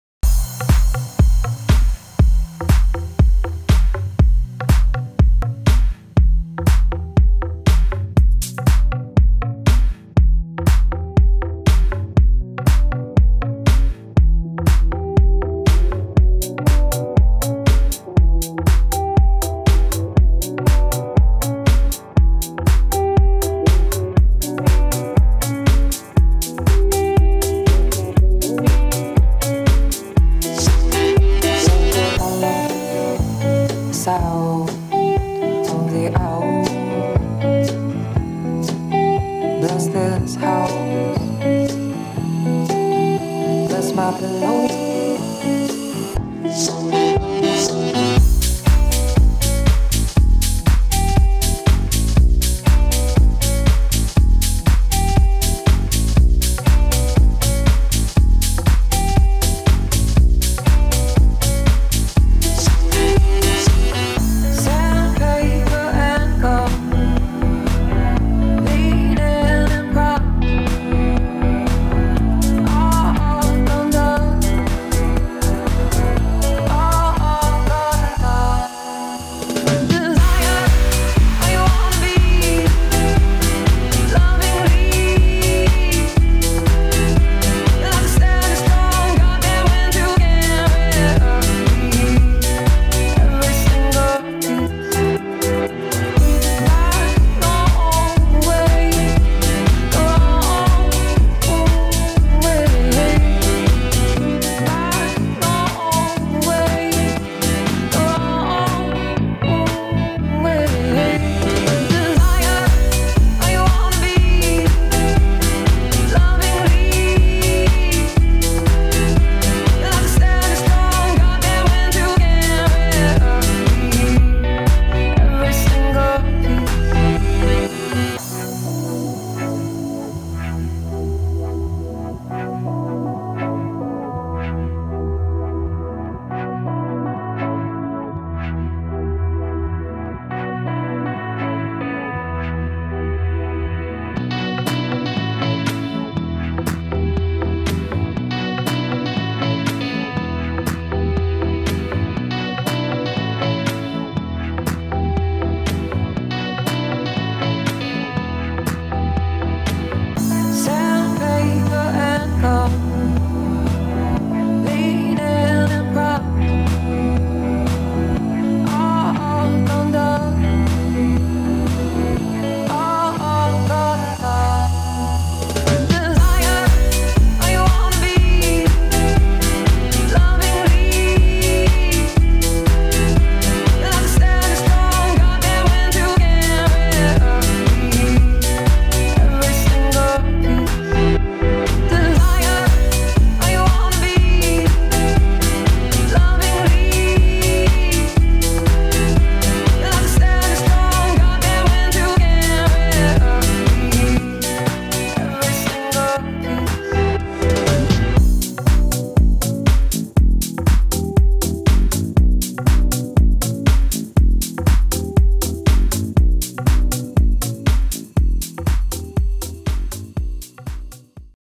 3Q summer Party by Zhero beach 17-05-15
video evento presentación nº20+2 revista 3Q social mag Abril 2015
Zhero beach club III opening party con la colaboración de y